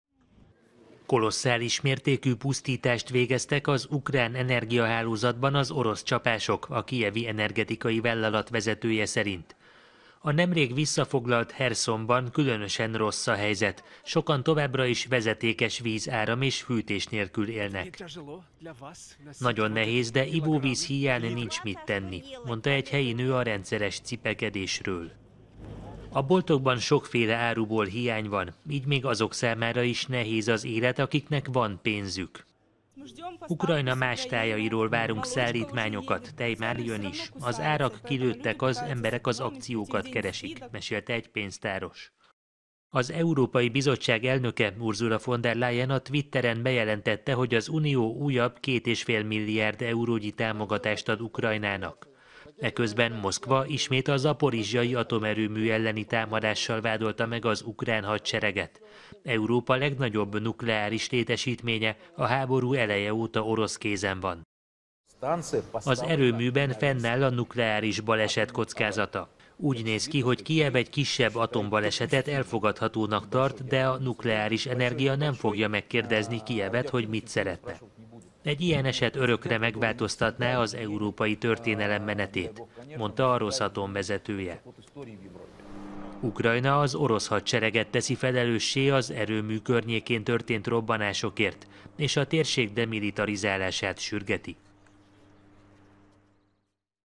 Kolosszálisnak nevezte a pusztítást az ukrán energiacég vezetője
A háborúzó felek ismét egymást vádolják a zaporizzsjai atomerőmű környékének bombázásával.